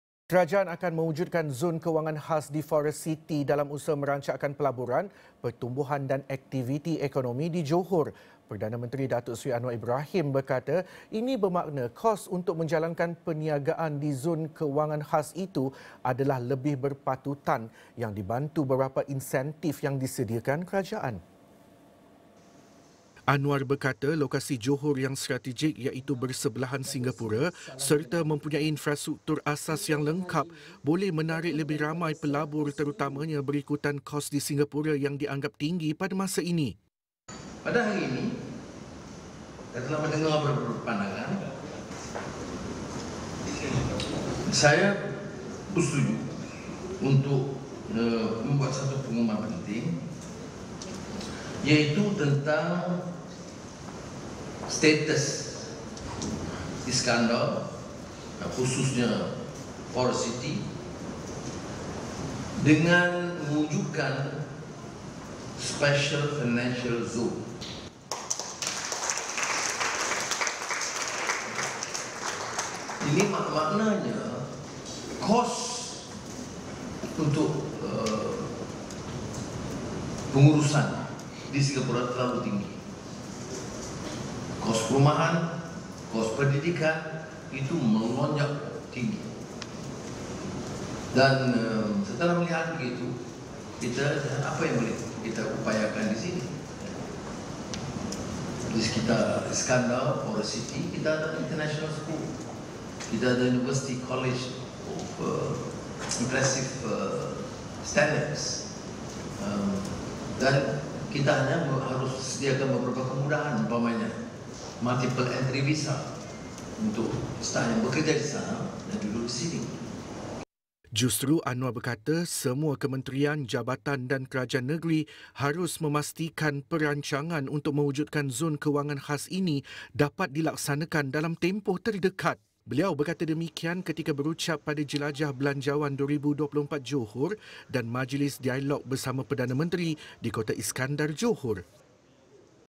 Zon kewangan khas diwujudkan di Forest City - PM
Kerajaan akan mewujudkan zon kewangan khas di Forest City dalam usaha merancakkan pelaburan, pertumbuhan dan aktiviti ekonomi di Johor.